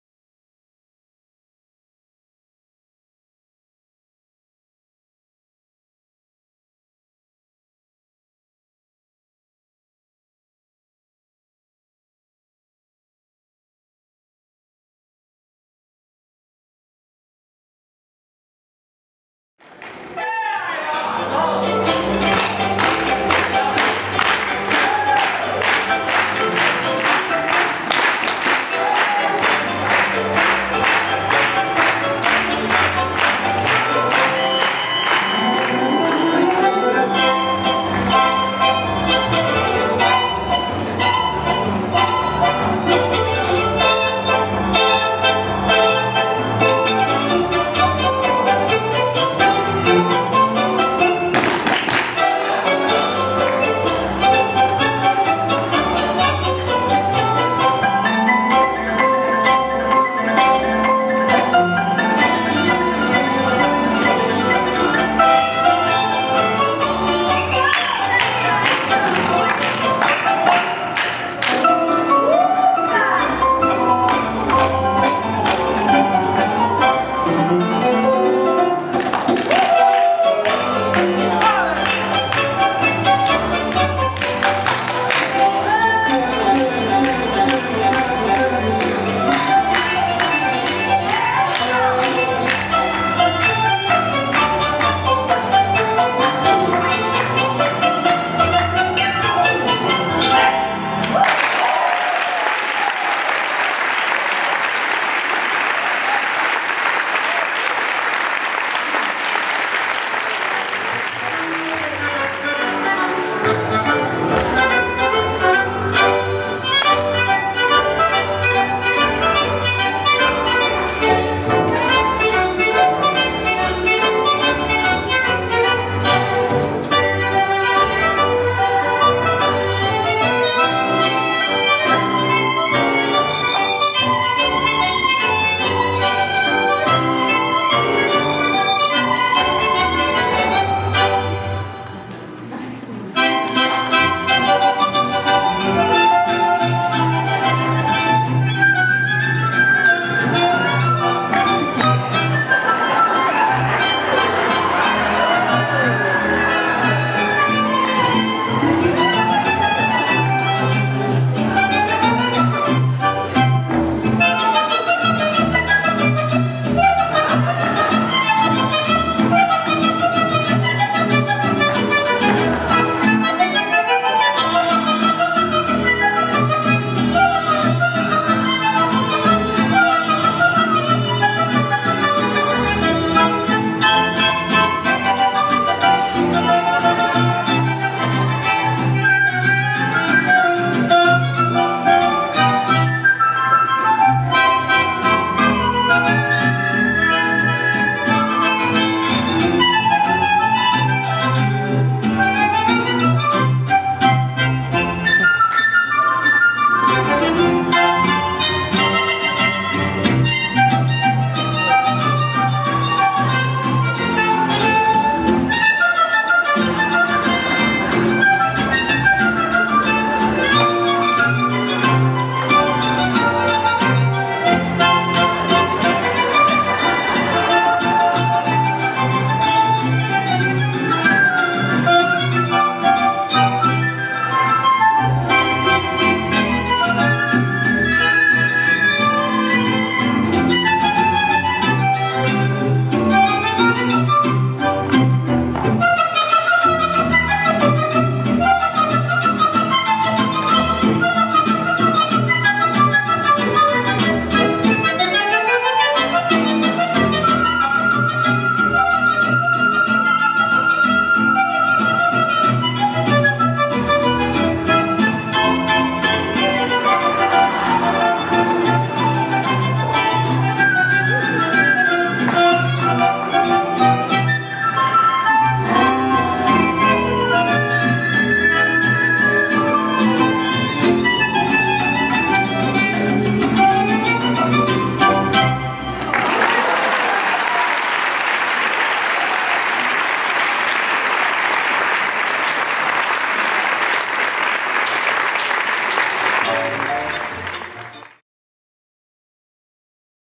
BCTango July 9, 2011 Main Gala Milonga Show.mov
BCTango July 9, 2011 Main Gala Presented by Gabriela Rojo - Miriam Larici, Leonardo Barrionuevo, Elizabeth Roccella, and Claudio Otero - Milonga Show.